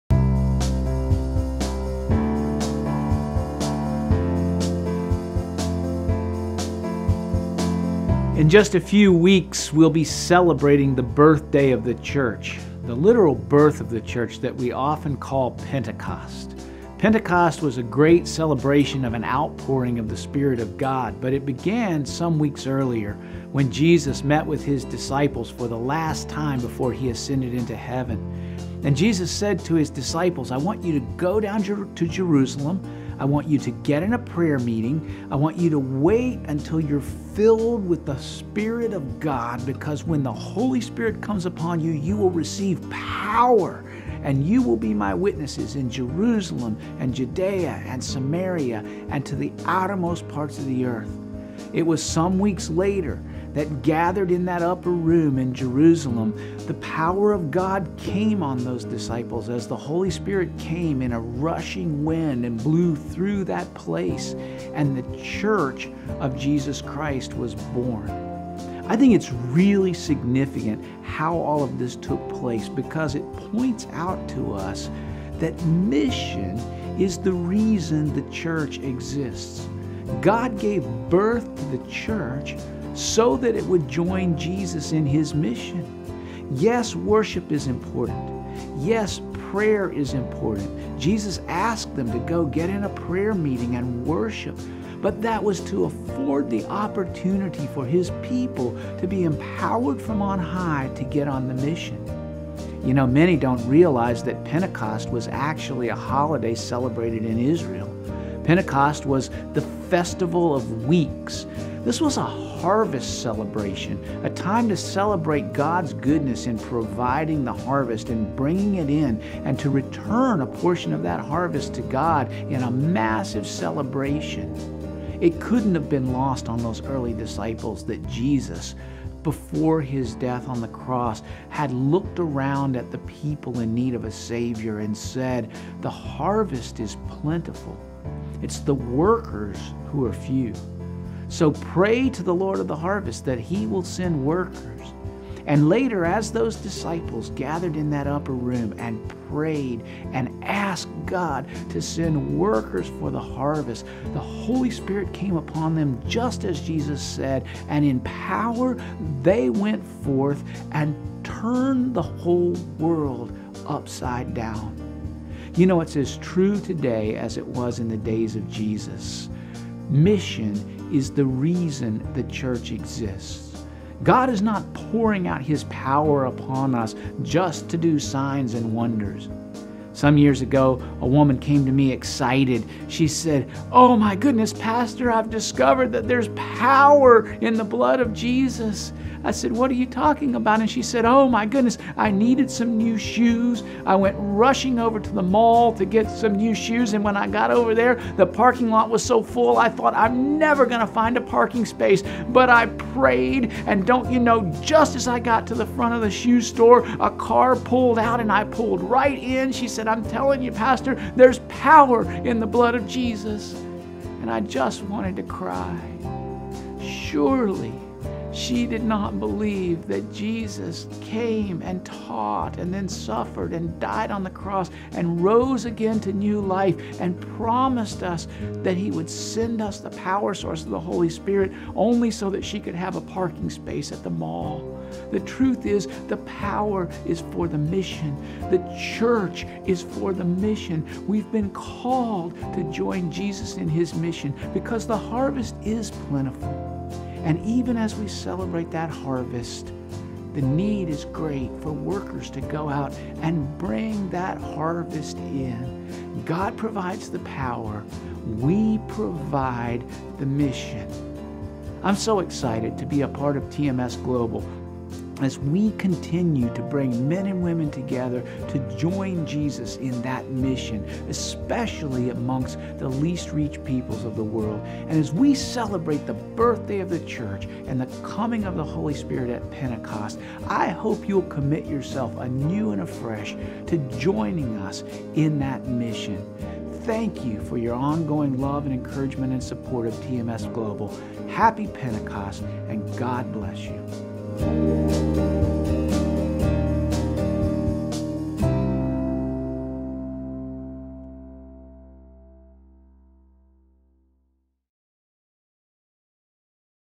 The true meaning of Pentecost
When Jesus ascended into heaven and directed the disciples to go and make disciples, He gave the Church her mission. President Wilkins discusses the connection between Jesus’ command and Pentecost, and what it means for each of us today.